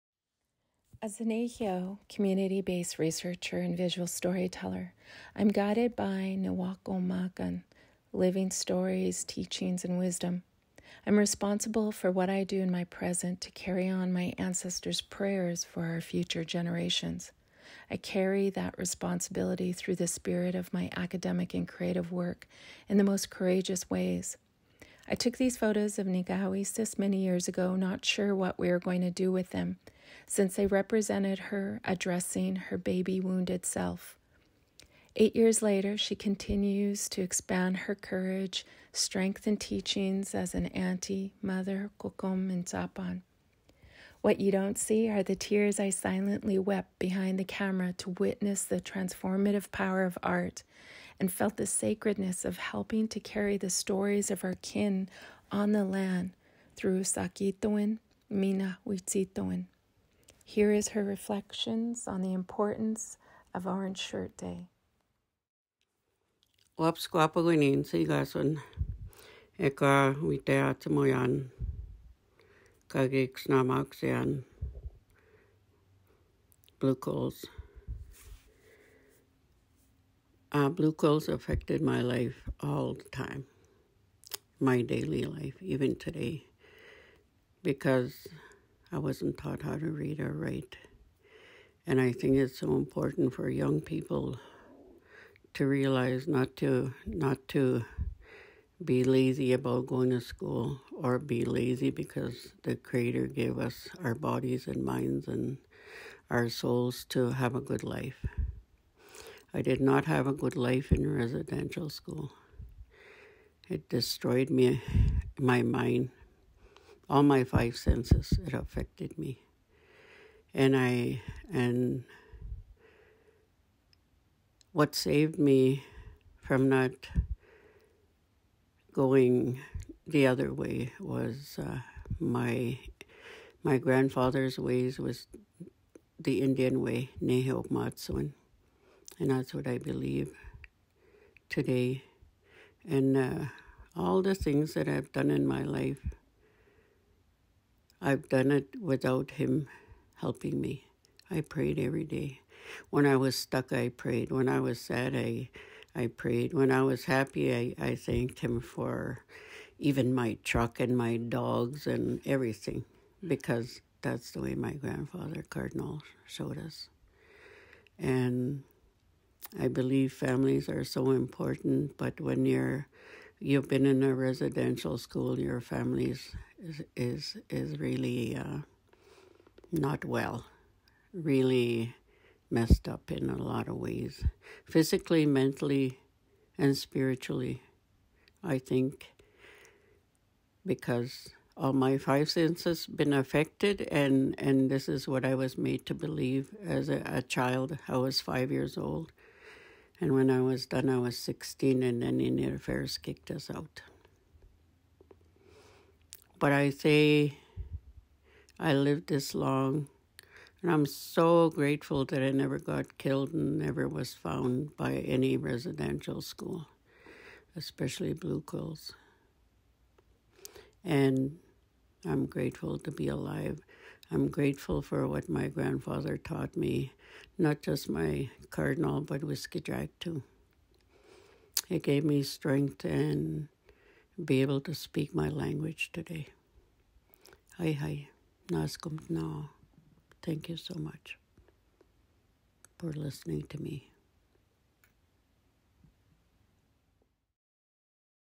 White Flower's Truth - National Day for Truth and Reconciliation
September 30 is National Day for Truth and Reconciliation. Many of our kitêhêyahak share, "We must hear the truth followed by action."

As a nêhiyaw visual storyteller and community-based researcher, as well as I am a descendant of generations of courageous matriarchs who attended  Blue Quills Indian Residential School (IRS). I am reminded every day of the impacts of IRS through my own disconnections and hard work of reconnecting to language, songs, ceremonies, and cultural teachings. I give gratitude daily for all the incredible nêhiyawak who are sharing healing and teaching stories, including nikawiysis Wapiskawapakwaniy.

These photos were taken in 2015 during a beauty retreat that I hosted with another artist. I was researching beauty at the time and photographing the ayisiniwak in their element of beauty, but nikawiysis wanted me to photograph her as her 5-year-old self when she was sent to Blue Quills IRS. During the photograph session, nikawiysis brought all the supplies she needed to replicate her 5-year-old self. This was a critical learning moment as I viewed her from behind the camera lens witnessing a transformative moment of reclamation of spirit. nikawiysis illuminated such fortitude in acknowledging and nurturing the child within who suffered so much from government-funded church-run schools.

Here is her reflection.

Photographs taken ion 2015 by Lana Whiskeyjack